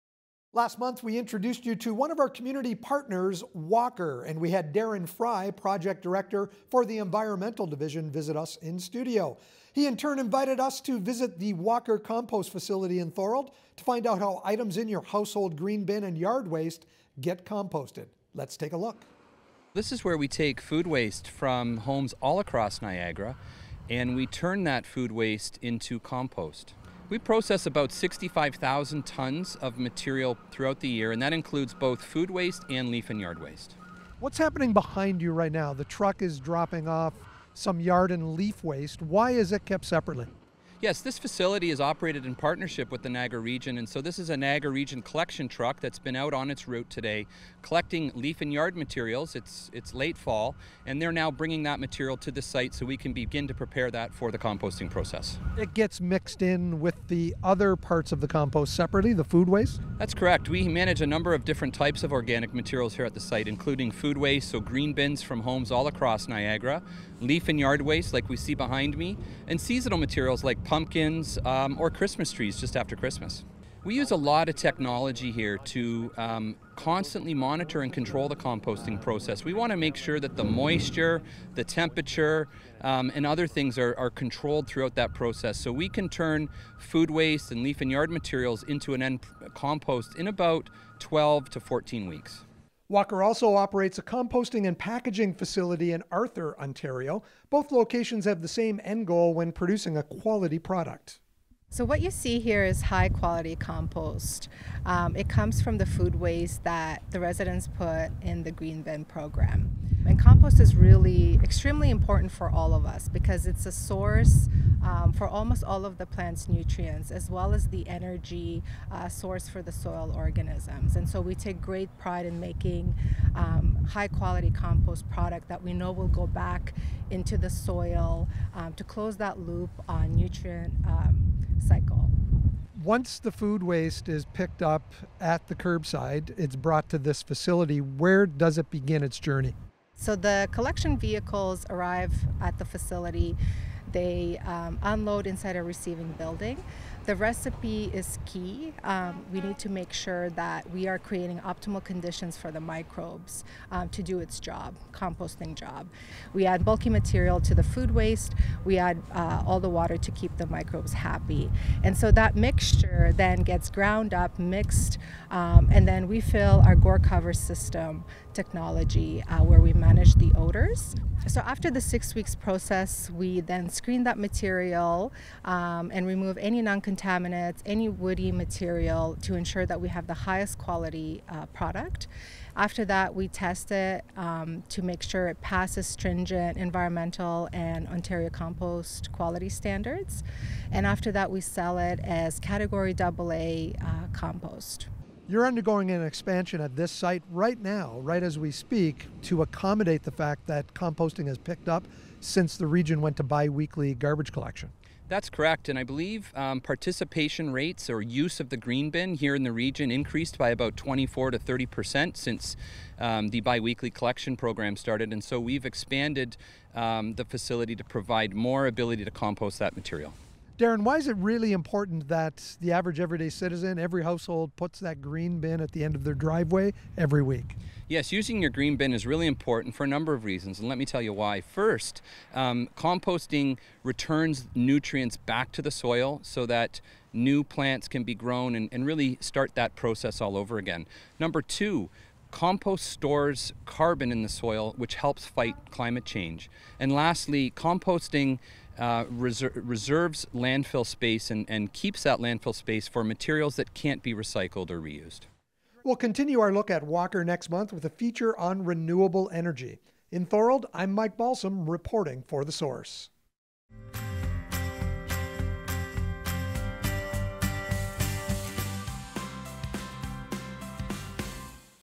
Walker - Episode 2: Composting
Mike Balsom visits with Darren Fry at the Walker Campus to learn more about Composting. Walker’s 12-acre composting facility in Thorold, Ontario utilizes the patented GORE® technology to divert over 65,000 tonnes of organic materials from landfill annually.